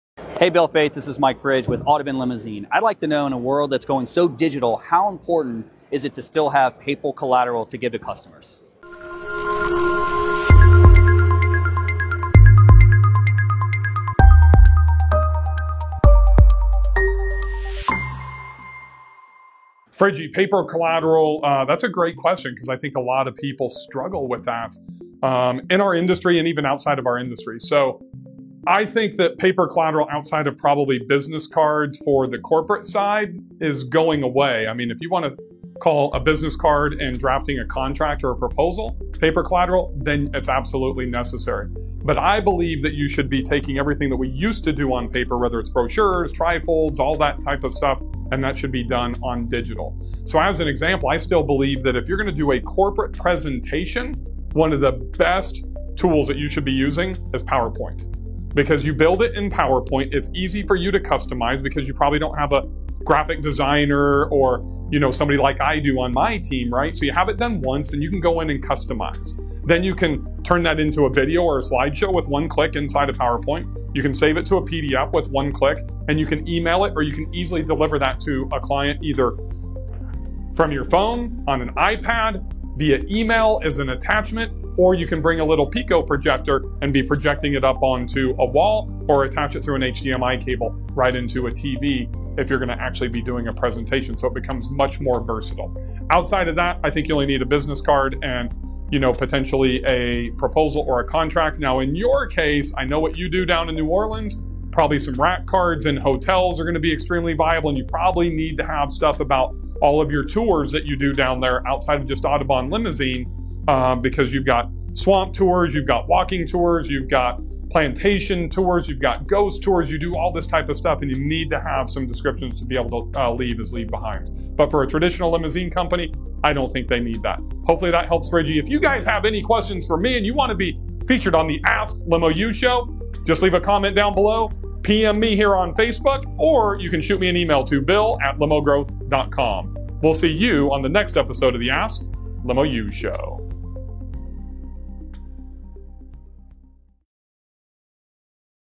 THE ASK LIMO U SHOW: Episode #119
Michael Fridge of Audubon Limousine in New Orleans ⚜️ wants to know how important paper collateral is for customers in a world gone digital 📲

Simply put? 🧐

✌🏼Outside of business cards, paper collateral is on it's way out ✌🏼

Take everything you’re used to doing on paper 📝 and convert it to digital! 💻

If you’re giving a business proposal 📈 utilize PowerPoint! It’s easy to design and perfect for customization! ⚙️

Have you made the permanent switch to digital? How’s the switch fared for you so far? 👇👇 See Less